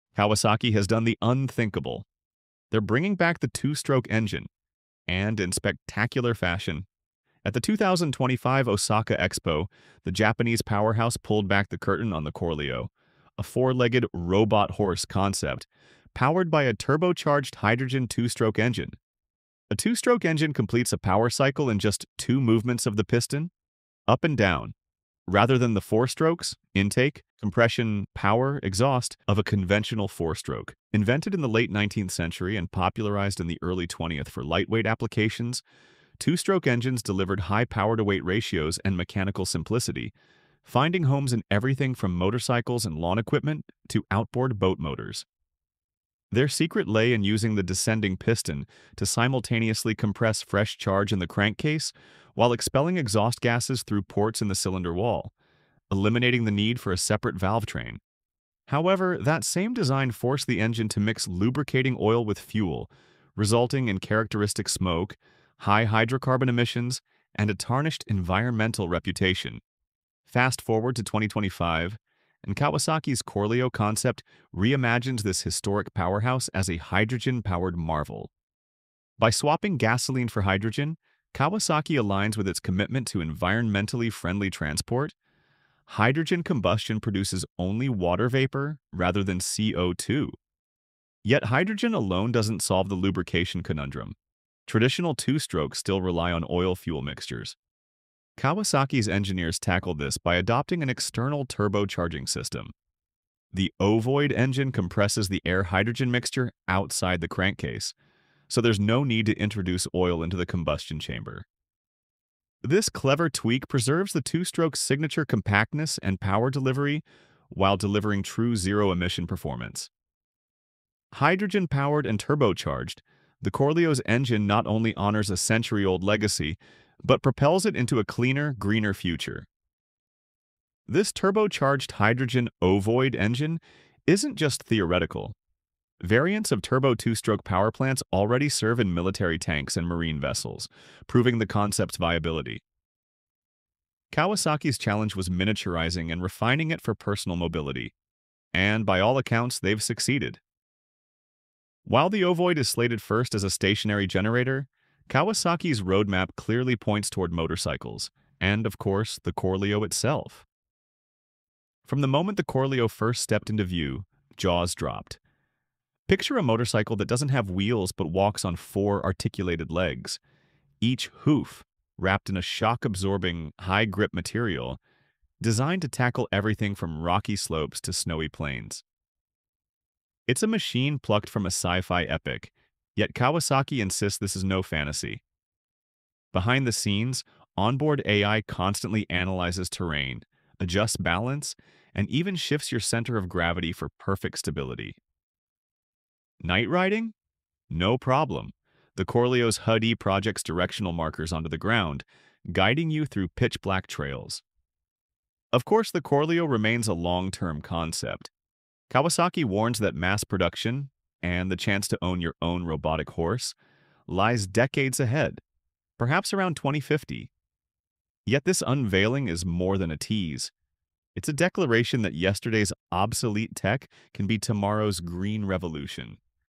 KAWASAKI'S 2025 2-STROKE ENGINE CHANGES EVERYTHING!
Kawasaki Heavy Industries has unveiled "CORLEO," a four-legged bionic robot powered by a hydrogen fuel cell system, showcasing future-ready mobile platform technologies.